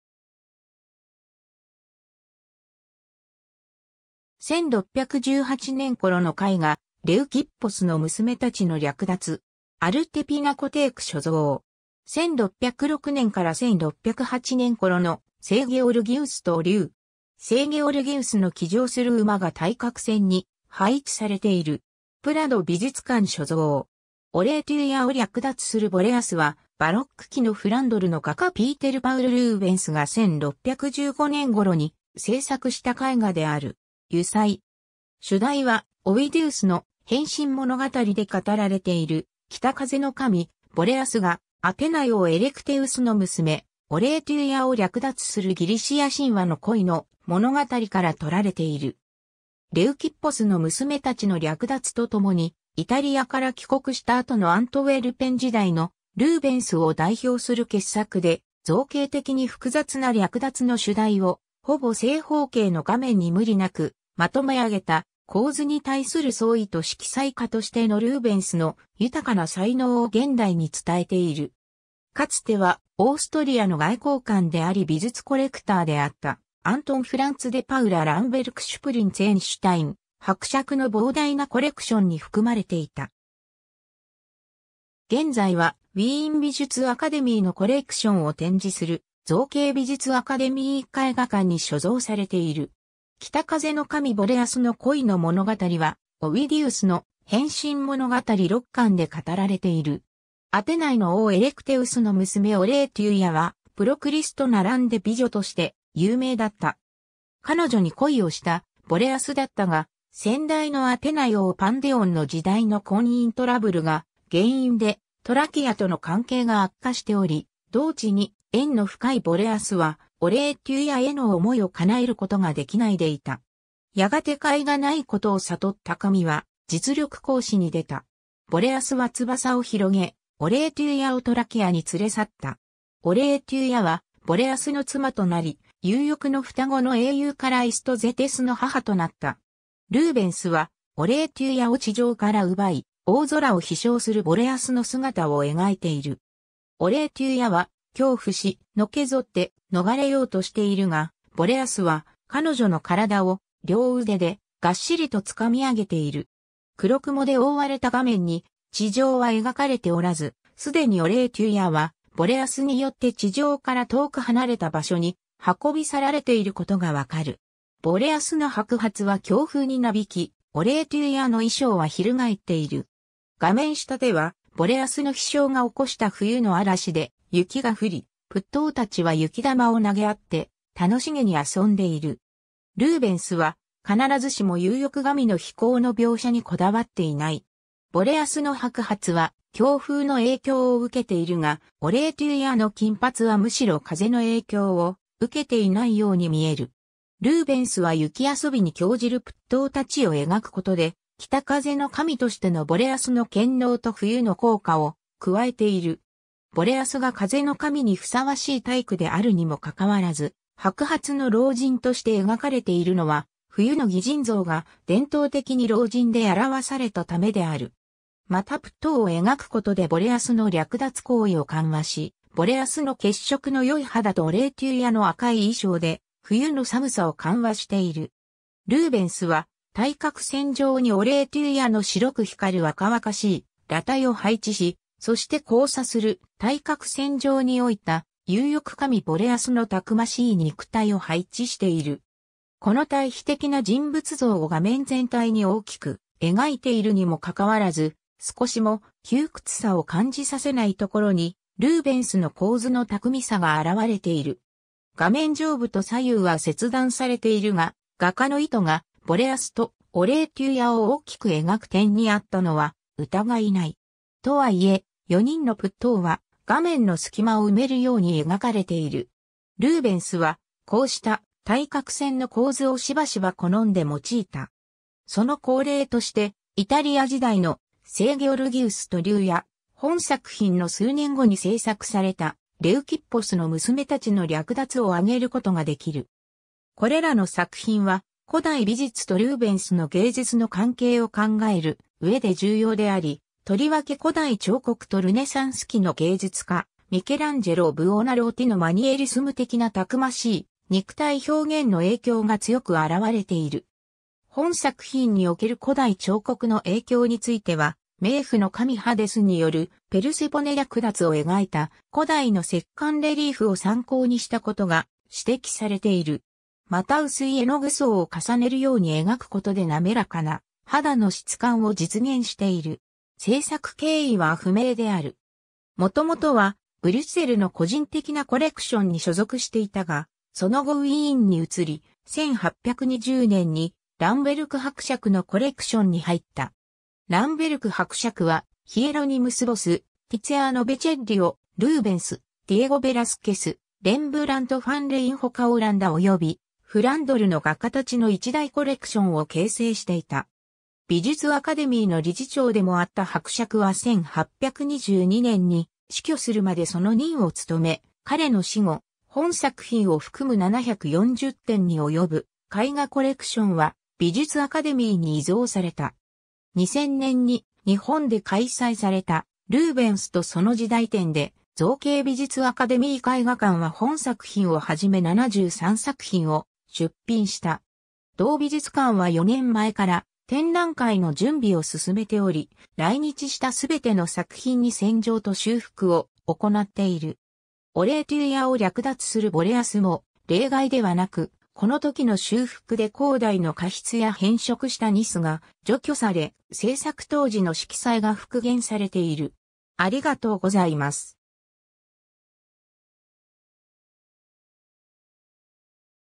オレイテュイアを略奪するボレアス
ピーテル・パウル・ルーベンスの作品
1618年頃の絵画『レウキッポスの娘たちの略奪』。
アルテ・ピナコテーク所蔵。
 1606年から1608年頃の『聖ゲオルギウスと竜』。
聖ゲオルギウスの騎乗する馬が対角線に配置されている。
プラド美術館所蔵。
 『オレイテュイアを略奪するボレアス』（独: Boreas entführt Oreithya, 英: The Rape of Orithyia by Boreas）は、バロック期のフランドルの画家ピーテル・パウル・ルーベンスが1615年頃に制作した絵画である。
油彩。
主題はオウィディウスの『変身物語』で語られている北風の神ボレアスがアテナイ王エレクテウスの娘オレイテュイアを略奪するギリシア神話の恋の物語から取られている。
 『レウキッポスの娘たちの略奪』（The Rape of the Daughters of Leucippus）とともにイタリアから帰国した後のアントウェルペン時代のルーベンスを代表する傑作で、造形的に複雑な略奪の主題をほぼ正方形の画面に無理なくまとめ上げた、構図に対する創意と色彩家としてのルーベンスの豊かな才能を現代に伝えている。
かつてはオーストリアの外交官であり美術コレクターであったアントン・フランツ・デ・パウラ・ランベルク＝シュプリンツェンシュタイン伯爵の膨大なコレクションに含まれていた。
現在はウィーン美術アカデミーのコレクションを展示する造形美術アカデミー絵画館に所蔵されている。
 北風の神ボレアスの恋の物語はオウィディウスの『変身物語』6巻で語られている。
アテナイの王エレクテウスの娘オレイテュイアはプロクリスと並んで美女として有名だった。
彼女に恋をしたボレアスだったが、先代のアテナイ王パンディオンの時代の婚姻トラブルが原因でトラキアとの関係が悪化しており、同地に縁の深いボレアスはオレイテュイアへの思いを叶えることができないでいた。
やがて甲斐がないことを悟った神は実力行使に出た。
ボレアスは翼を広げ、オレイテュイアをトラキアに連れ去った。
オレイテュイアはボレアスの妻となり、有翼の双子の英雄カライスとゼテスの母となった。
 ルーベンスはオレイテュイアを地上から奪い、大空を飛翔するボレアスの姿を描いている。
オレイテュイアは恐怖し、のけぞって逃れようとしているが、ボレアスは彼女の身体を両腕でがっしりと掴み上げている。
黒雲で覆われた画面に地上は描かれておらず、すでにオレイテュイアはボレアスによって地上から遠く離れた場所に運び去られていることが分かる。
ボレアスの白髪は強風になびき、オレイテュイアの衣装は翻っている。
画面下ではボレアスの飛翔が起こした冬の嵐で雪が降り、プットーたちは雪玉を投げ合って楽しげに遊んでいる。
ルーベンスは必ずしも有翼神の飛行の描写にこだわっていない。
ボレアスの白髪は強風の影響を受けているが、オレイテュイアの金髪はむしろ風の影響を受けていないように見える。
 ルーベンスは雪遊びに興じるプットーたちを描くことで、北風の神としてのボレアスの権能と冬の効果を加えている。
ボレアスが風の神にふさわしい体躯であるにもかかわらず、白髪の老人として描かれているのは、冬の擬人像が伝統的に老人で表されたためである。
またプットーを描くことでボレアスの略奪行為を緩和し、ボレアスの血色の良い肌とオレイテュイアの赤い衣装で冬の寒さを緩和している。
 ルーベンスは対角線上にオレイテュイアの白く光る若々しい裸体を配置し、そして交差する対角線上に老いた有翼神ボレアスの逞しい肉体を配置している。
この対比的な人物像を画面全体に大きく描いているにもかかわらず、少しも窮屈さを感じさせないところにルーベンスの構図の巧みさが表れている。
画面上部と左右は切断されているが、画家の意図がボレアスとオレイテュイアを大きく描く点にあったのは疑いない。
とはいえ、4人のプットーは画面の隙間を埋めるように描かれている。
ルーベンスはこうした対角線の構図をしばしば好んで用いた。
その好例としてイタリア時代の『聖ゲオルギウスと竜』（Saint George and the Dragon, 1606年-1608年頃）や、本作品の数年後に制作された『レウキッポスの娘たちの略奪』を挙げることができる。
これらの作品は古代美術とルーベンスの芸術の関係を考えるうえで重要であり、とりわけ古代彫刻とルネサンス期の芸術家ミケランジェロ・ブオナローティのマニエリスム...